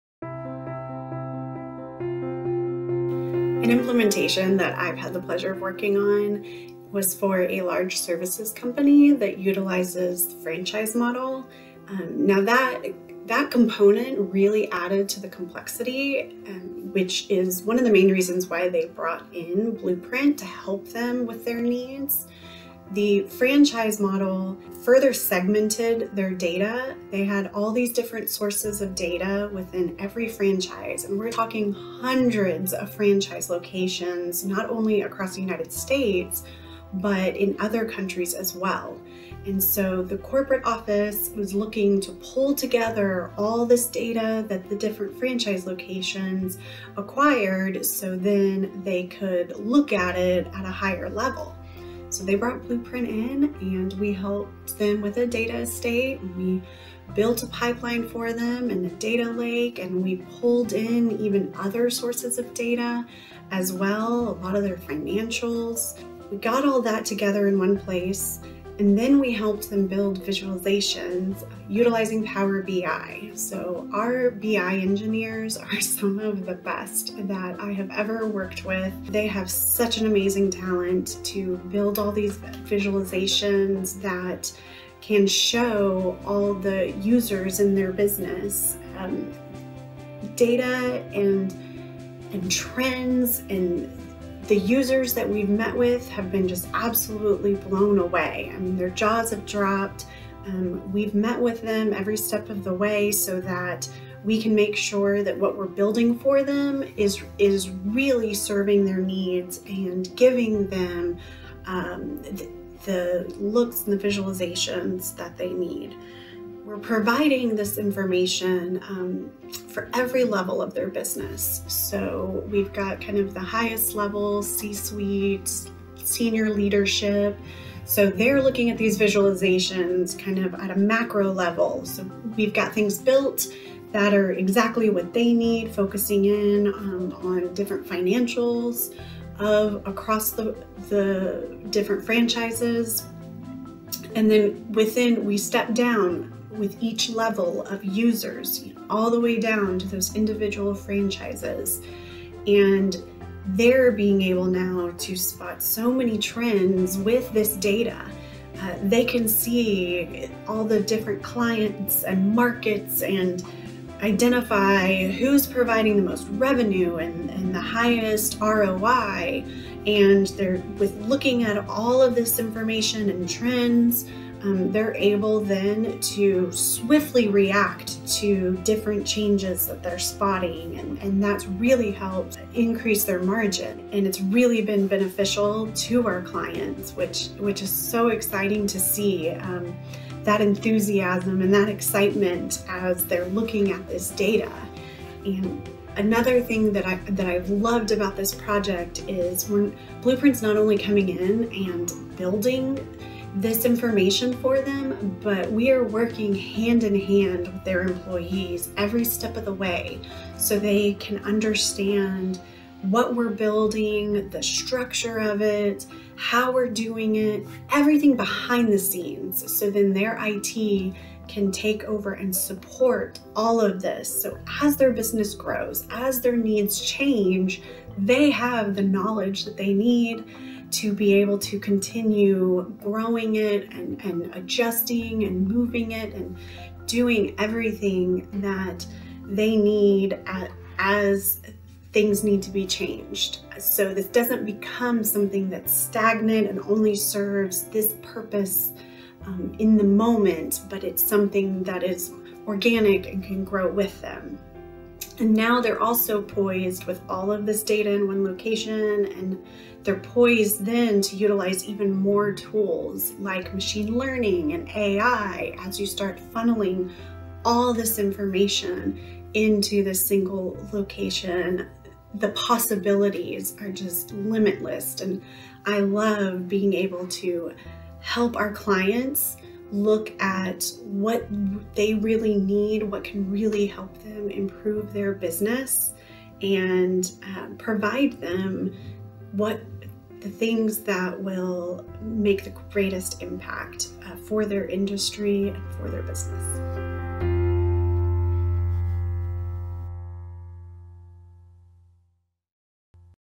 Blueprint Implemented - Data platform modernization
We've talked a lot about the benefits of our Modern Data Estate solution, but today we want to detail some of the tangible results it can help an organization achieve. Take 5 minutes to listen to Blueprint Delivery Manager Lindsey Pyron explain how we implemented this solution with one of our clients and the improvements they've experienced since.